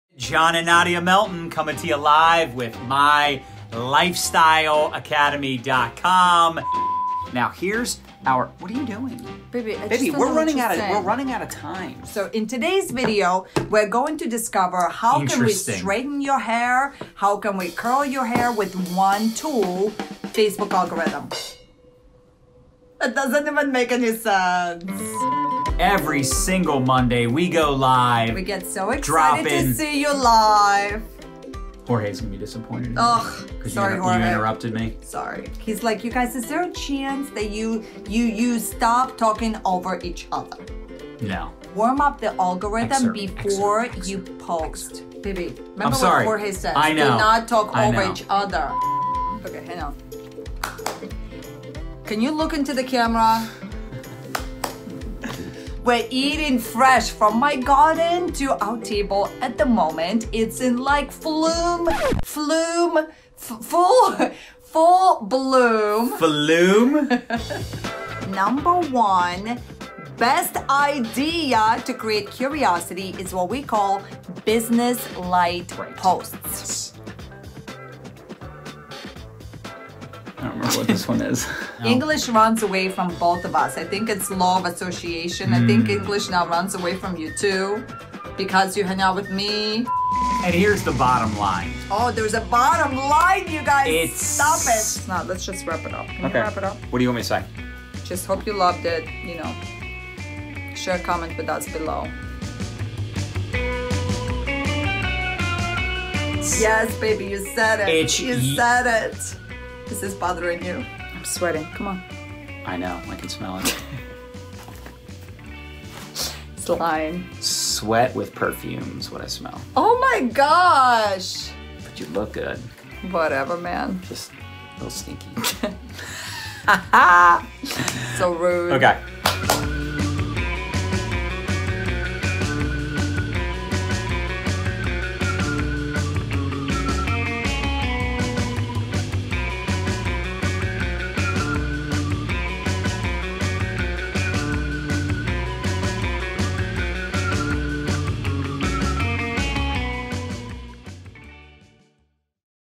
Bloopers Collection #1 - John & Nadya Melton
Some people call it "making mistakes", we call it "HAVING FUN AT WORK"!
This is our best blooper reel so far 🤣 so don't laugh at us, LAUGH WITH US!